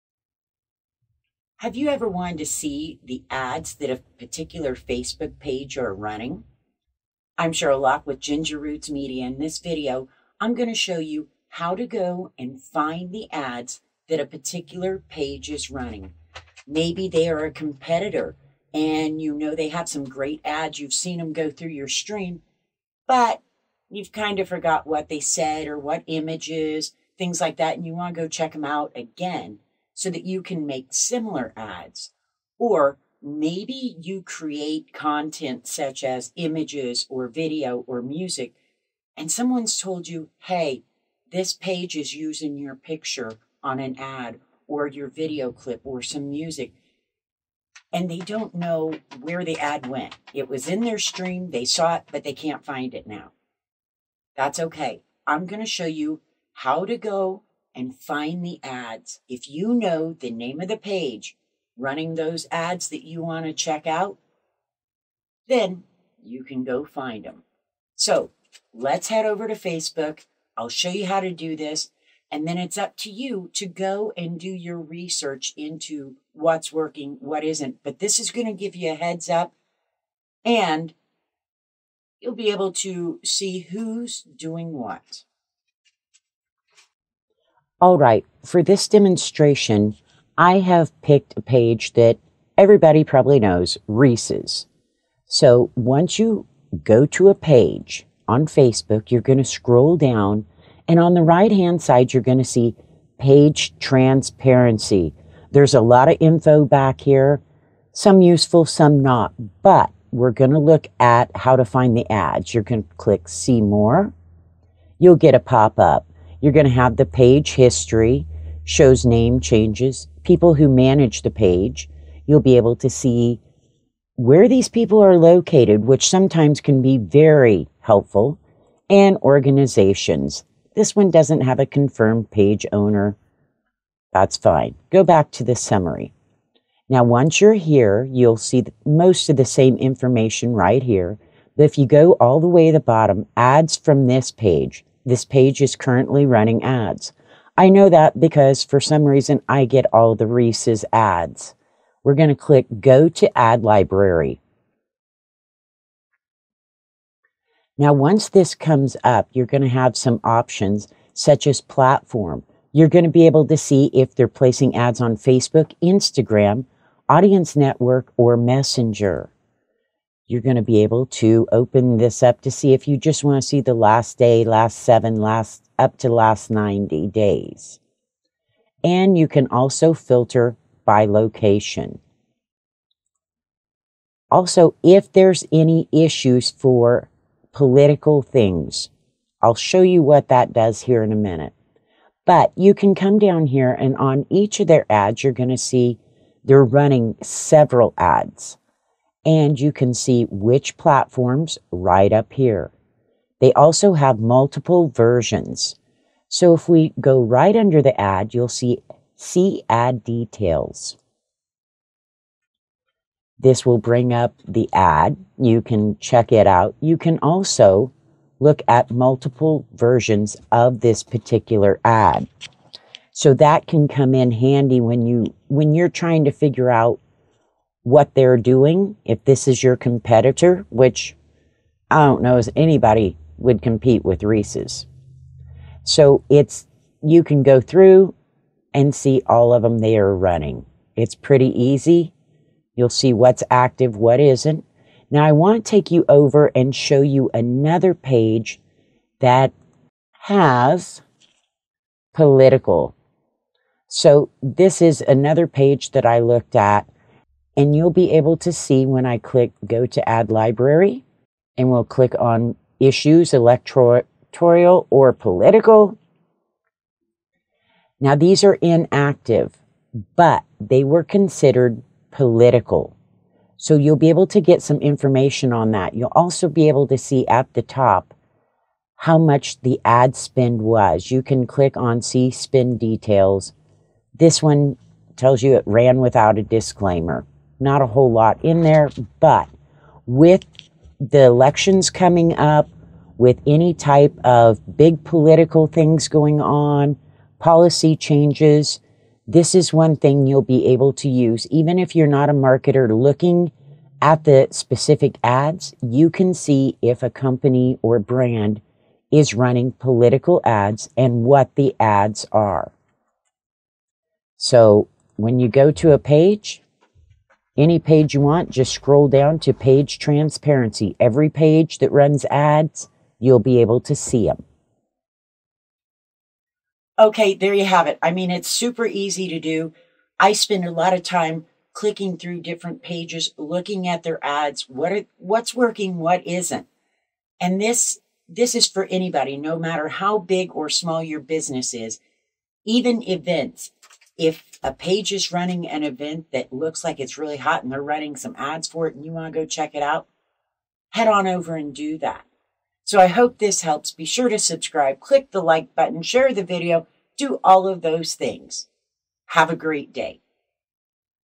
How To See Competitors Facebook Ads for Free
Do you want to see the ads your competitors are running on Facebook?
Are they also running ads on Instagram?
This is a super easy way for you to find the ads that any Facebook Page is running.
You can then get ideas for your own Facebook advertising or see if they are running any political ads.

My recommended video gear

Sheryl Loch
Ginger Roots Media